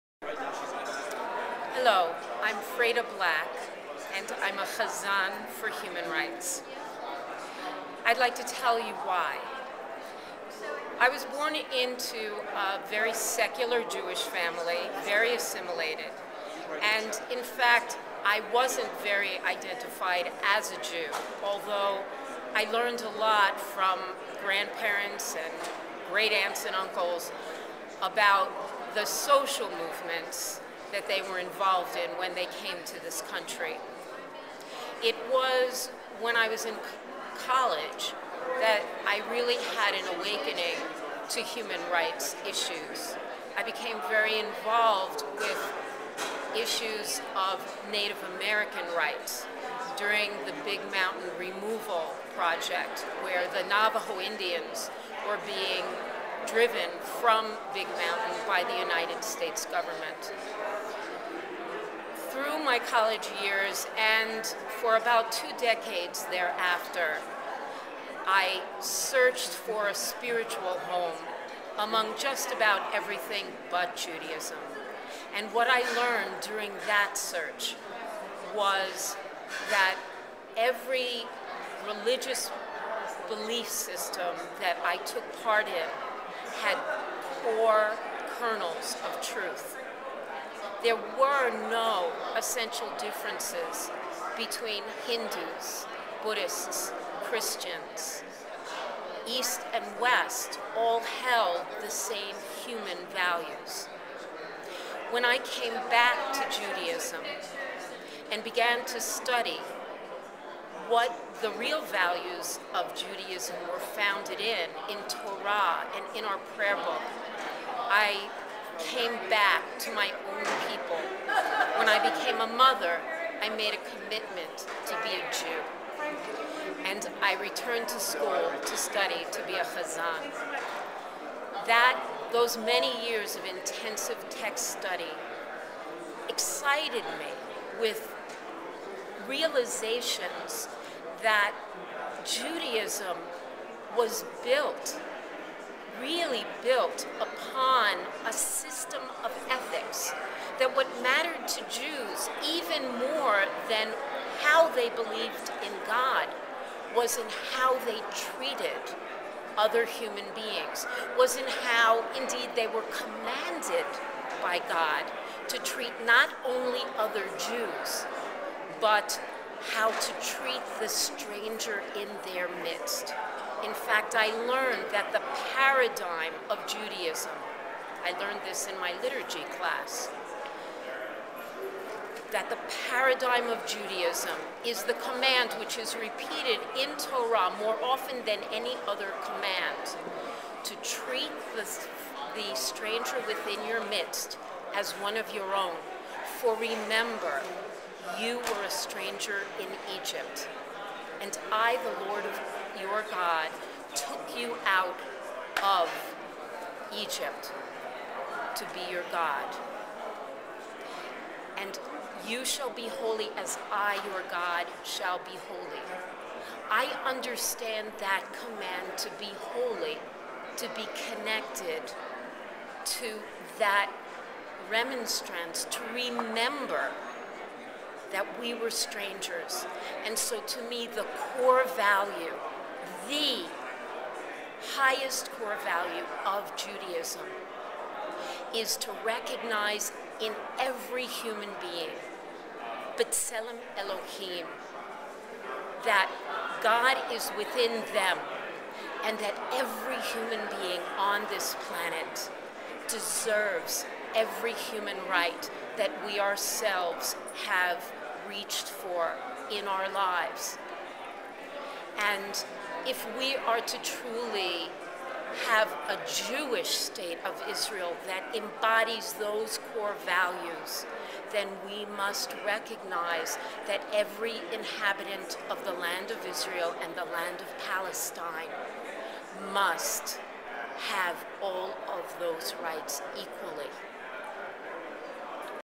"I Am a Rabbi for Human Rights" - Cantor Freyda Black
Rabbis for Human Rights -- North America (RHR-NA) is an organization of rabbis from all streams of Judaism that acts on the Jewish imperative to respect and protect the human rights of all people. Cantor Freyda Black tells us why she supports "Rabbis for Human Rights."